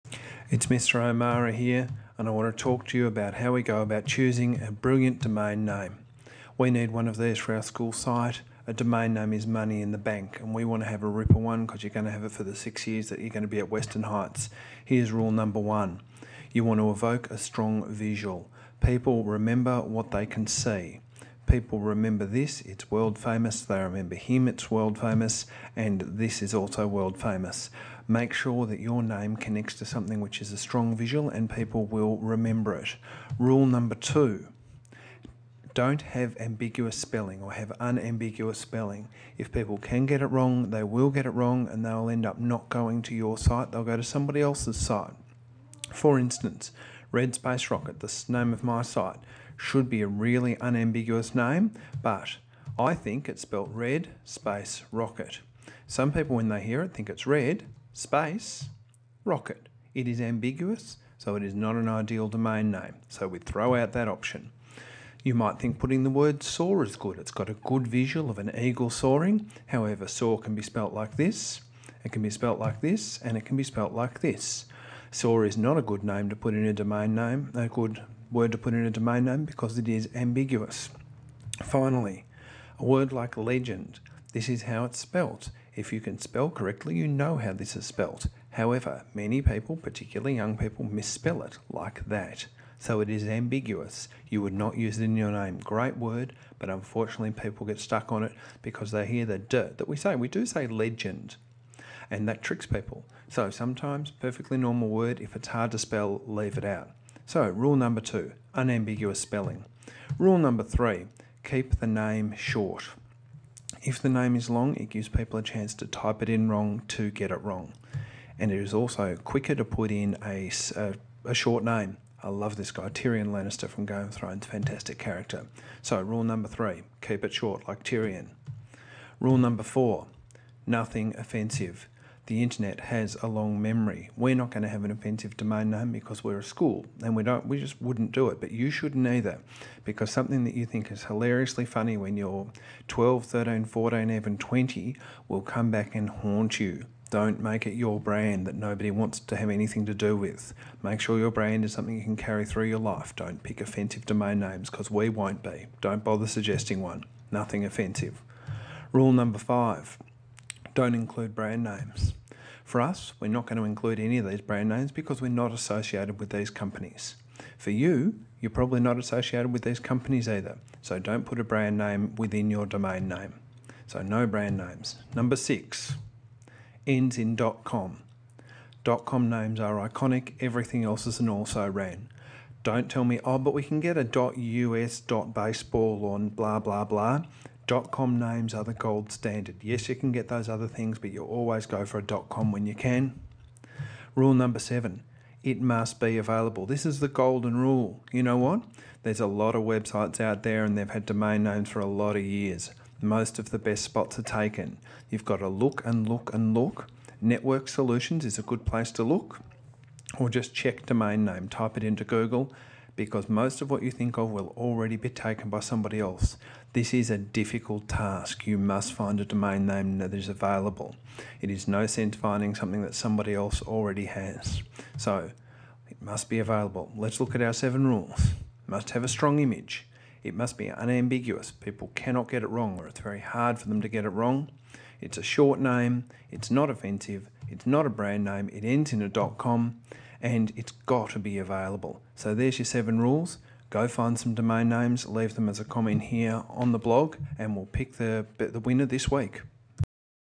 Choosing Our Brilliant Domain Name
Here are the rules that we will follow as we create the domain name for our new year-level site.